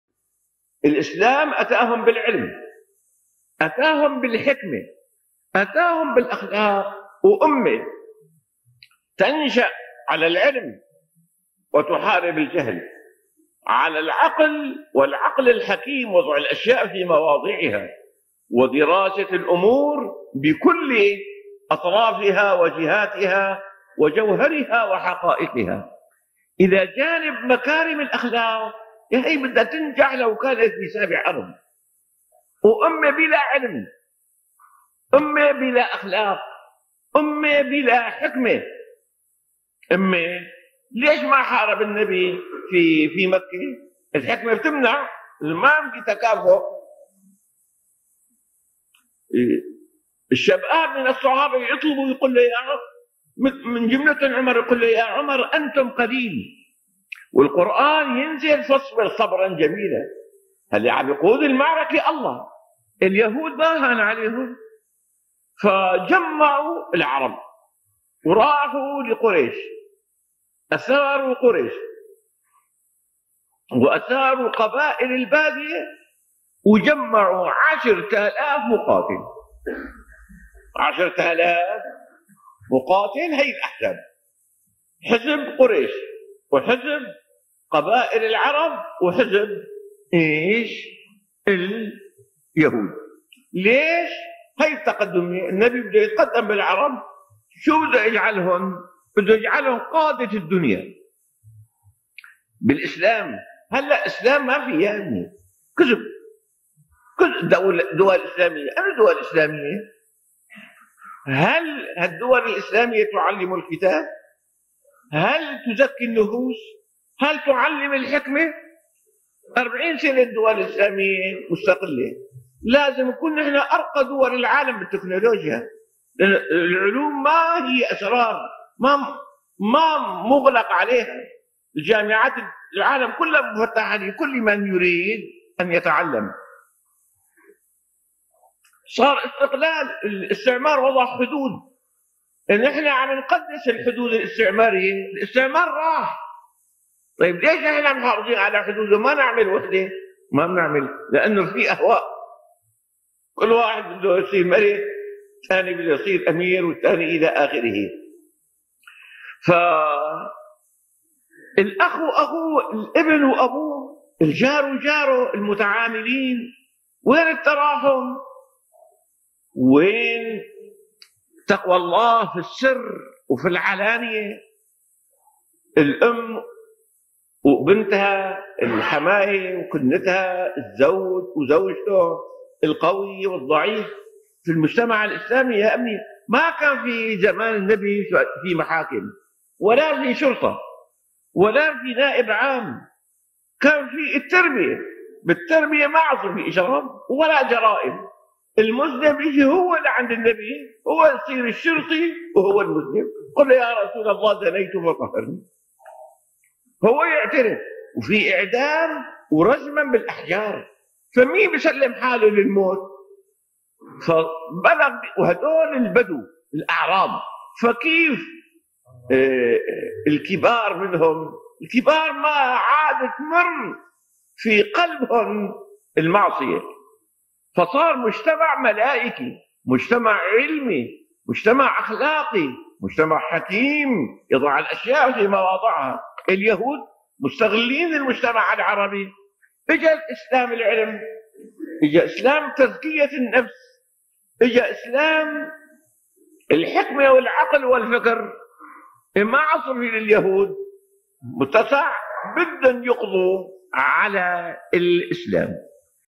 No advancement for Arabs without Islam
The confederates 3

18/04/1986

الإسلام أتاهم بالعلم
أتاهم بالحكمة أتاهم بالأخلاق
Islam brought knowledge,
wisdom and morals to the Arabs.
وأمة تنشأ على العلم وتحارب الجهل
على العقل والعقل الحكيم
A nation which fights against ignorance
 and is based on knowledge and wisdom
وضع الأشياء في مواضعها
ودراسة الأمور بكل أطرافها وجهاتها
which is putting things in their right
places and studying things fully from
وجوهها وحقائقها.
إذا جانب مكارم الأخلاق
هي بدها تنجح ولو بسابع أرض.
all sides and shows good morals
will surely succeed no matter where it is.
وأمة بلا علم
أمة بلا أخلاق
  أمة بلا حكمة.
However, a nation without knowledge,
morals or wisdom will never succeed.
ليش ما حارب النبي في مكة؟
Why didn't Prophet Muhammad fight
 his enemies when he was in Mecca?
الحكمة تمنع
Because wisdom prevented
 him from doing so.
لأنه لا يوجد تكافؤ.
Because there was no equivalence
 between the two parties.
الشباب من الصحابة يطلبوا
Once some young companions including
Omar asked the Prophet to allow them
يقول له من جملتهم عمر،
to defend themselves
 against the pagans of Quraysh.
يقول له: "يا عمر أنتم قليل"
 However, the Prophet refused saying:
“O Omar! Your number is small.
والقرآن ينزل:
  "فاصبر صبر جميلا."
Then the following verse was revealed:
“So be patient, with a good patience.”
الذي يقود المعركة الله.
In fact, Allah was leading the battle.
اليهود ما هان عليهم
فجمعوا العرب وراحوا لقريش
Some Jewish tribes incited
some Arab tribes including Quraysh
أثاروا قريش وأثاروا قبائل البادية

وجمعوا 10000 مقاتل.
 and formed a coalition of 10000 fighters to uproot Islam.

  هي الأحزاب:
حزب قريش، وحزب قبائل
العرب وحزب اليهود.
The confederates were Quraysh,
some Arab and Jewish tribes.
ليش؟ هذه هي التقدمية.
النبي بده يتقدم بالعرب.
شو بده يجعلهم؟
The Prophet wanted to make the Arabs advanced. He wanted to make them
بده يجعلهم قادة الدنيا بالإسلام.
هلق إسلام ما في.
the leaders of the world by following Islam.
Nowadays, we don't have genuine Islam.
كذب، كل الدول الإسلامية
أنو دول إسلامية؟
All Islamic states are not
 practicing genuine Islam.
هل هالدول الإسلامية تعلم الكتاب؟
Do these Islamic states teach
the Quran as it should be taught?
هل تزكي النفوس؟
هل تعلم الحكمة؟
Do they really purify the inner selves?
Do they really teach wisdom?
أربعين سنة الدول الإسلامية مستقلة
لازم نكون
The Islamic states got their independence
a long time ago. Thus, they should be the most
نحن ارقى دول العالم في التكنولوجيا.
لأن العلوم ما هي أسرار
advanced countries in technology
because modern sciences are available.
ما مغلق عليها.
جامعات العالم مفتوحة لكل من يريد أن يتعلم.
  Universities are available all over
 the world to anyone who wants to learn.
صار استقلال
الاستعمار وضع حدود.
Colonialism imposed borderlines on
the Arab countries. However, after they got
نحن عم نقدس الحدود الاستعمارية.
their independence and colonial powers
went away, the Arab states have been
الاستعمار راح.
consecrating those borderlines till today.
طيب ليش نحن محافظين على حدوده؟
ما نعمل وحدة؟
Why do they still keep those borderlines?
Why don't they unite?
ما منعمل لأنه في أهواء. كل واحد بده يصير ملك
Because of personal interests and struggle for power

الثاني بده يصير أمير
والثالث الخ.
as everybody wants to be a king or a prince.

الأخ وأخوه،
الابن وأبوه
الجار وجاره
There is no mercy among brothers,
sons and fathers, neighbors and
المتعاملين، أين التراحم؟
أين تقوى الله في السر وفي العلانية؟
 people in daily dealings.
Where is God’s fear secretly and openly?
الأم وبنتها
Where is mercy among  mothers and daughters?

الحماية وكنتها،
Among mothers-in-law and their daughters-in-law?

الزوج وزوجته
القوي والضعيف.
Among husbands and wives?
Among the weak and the strong?
في المجتمع الإسلامي لم يكن هناك
زمان النبي ما كان في محاكم ولا في شرطة
There weren't courts, police or
 an Attorney General in the Islamic society
ولا في نائب عام.
كان في تربية، بالتربية ما عد في
  إجرام ولا جرائم.
during the time of the Prophet. There was Islamic education which prevented criminality.
المسلم بيجي هو لعند النبي
هو يصير االشرطي وهو المذنب.
If a Muslim committed a sin,
 he would behave like a policeman.
يقول يا رسول الله زنيت
He would come to the Prophet and say:
 “O Messenger of Allah! I committed adultery.
فطهرني
 Purify me.”
هو يعترف وفي إعدام رجما بالأحجار.
He would confess to the Prophet although
he knew that he would be stoned to death.
فمين يسلم حاله للموت؟
وهدول البدو الأعراب،
Who would surrender to death like that?
That was how the Arab Bedouins behaved.
فكيف الكبار منهم؟
الكبار ما عاد تمر في قلبهم المعصية،
What about their leaders?
Sinfulness wouldn't occur to them.
فصار مجتمع ملائكي، مجتمع علمي،
مجتمع أخلاقي، مجتمع حكيم
Thus, society became angelic, scientific, moralistic and wise which put things
يضع الأشياء في مواضعها.
in their right places.
اليهود مستغلين المجتمع العربي.
Before Islam, some Jewish tribes
Exploited the Arab community.